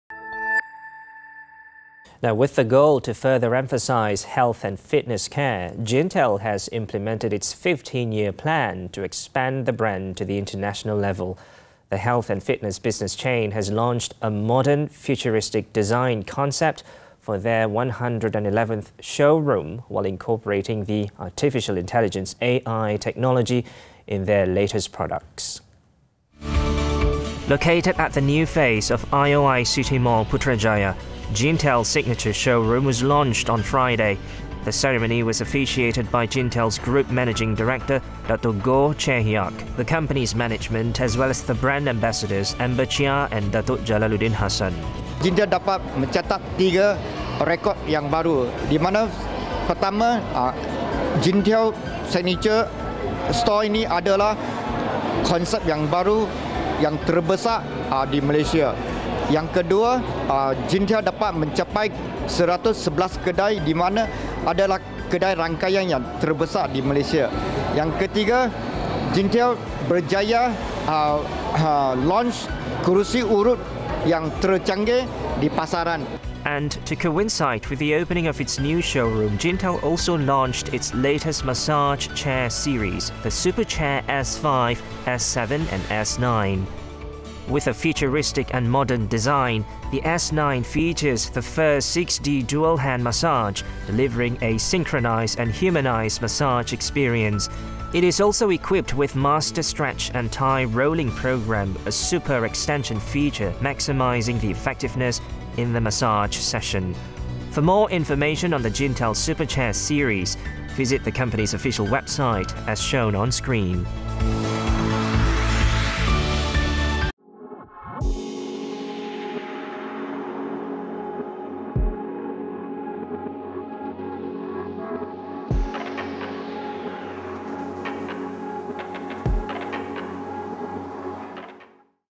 GINTELL | 111th Showroom With Modern Futuristic Design
With the goal to further emphasise health and fitness care, Gintell has implemented its 15-year plan to expand the brand to the international level.

The health and fitness business chain has launched a modern futuristic design concept for their 111th showroom, while incorporating the Artificial Intelligence (AI) technology in their latest products.